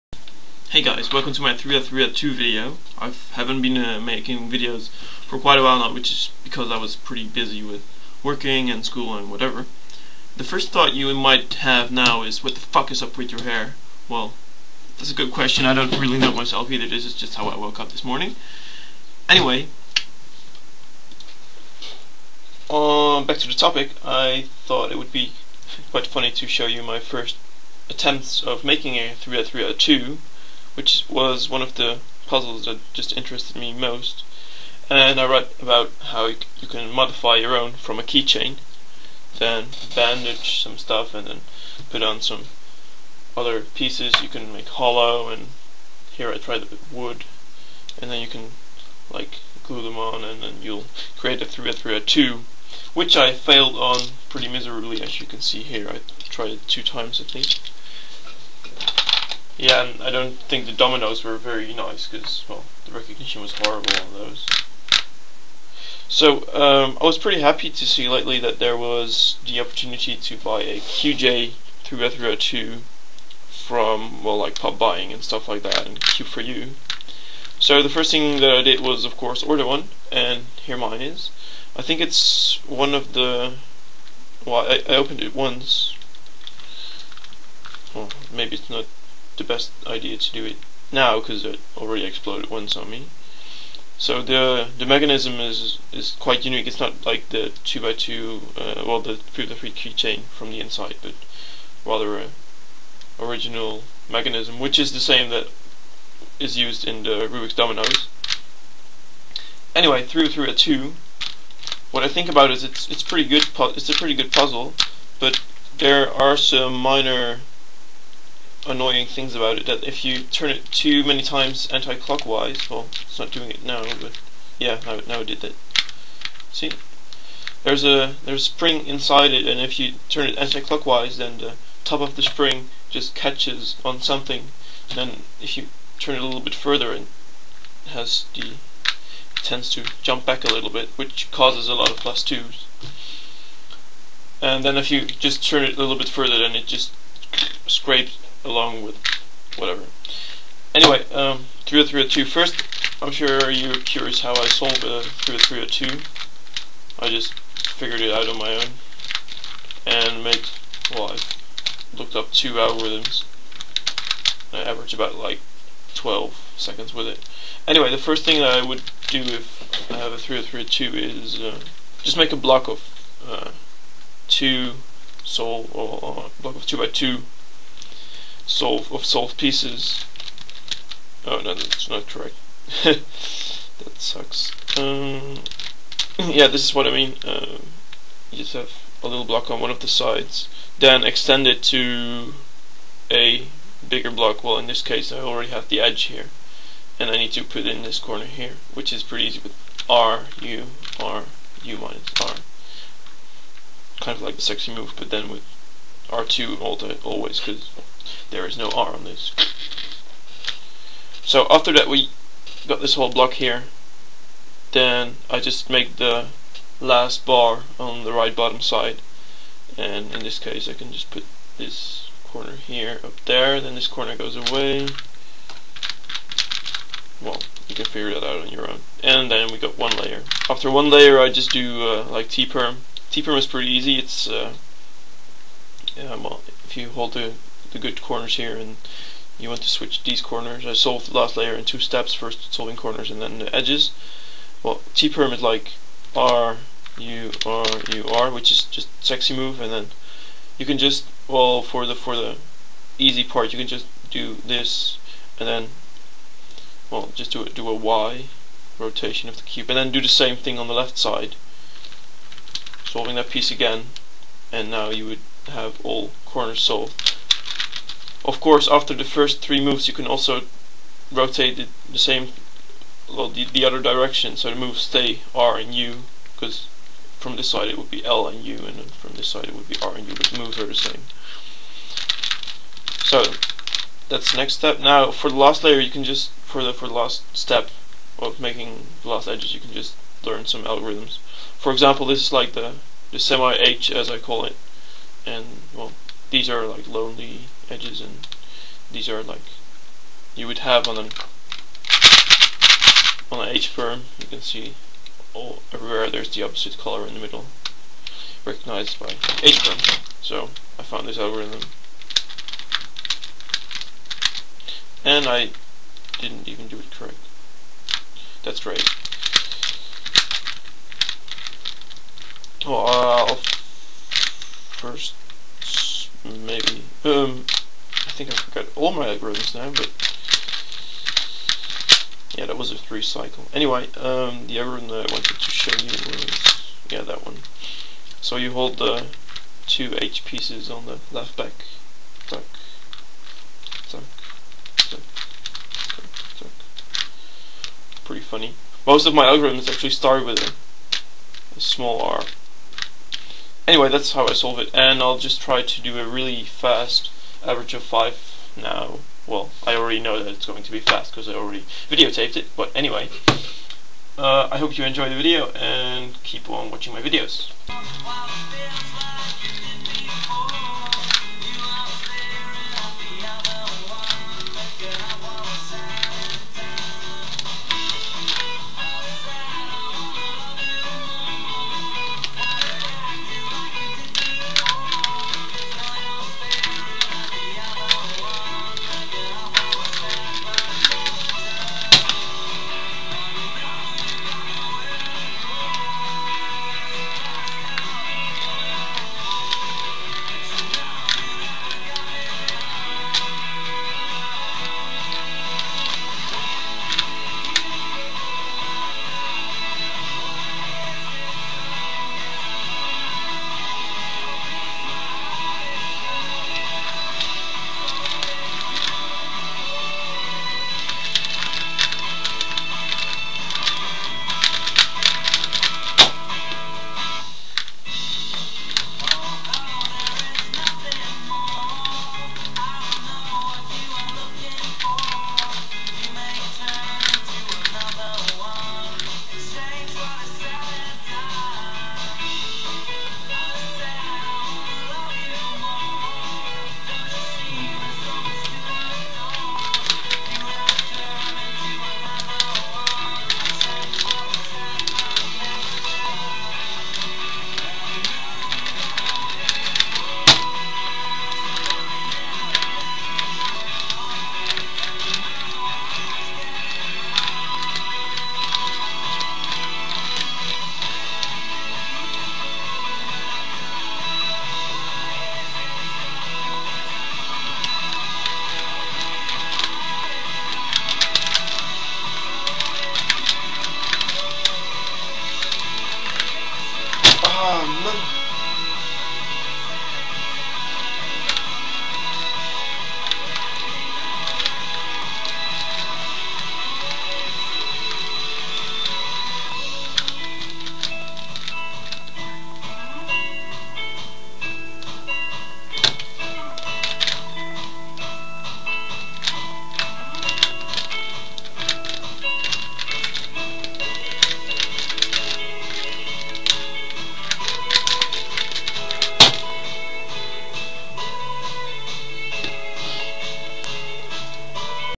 3x3x2 average of 5 10.24, and tutorial and review
Solving starts at: 6:23 !
Times for my average of 5: 9.61, (9.34), 11.76, 9.35, (13.13) = 10.24 average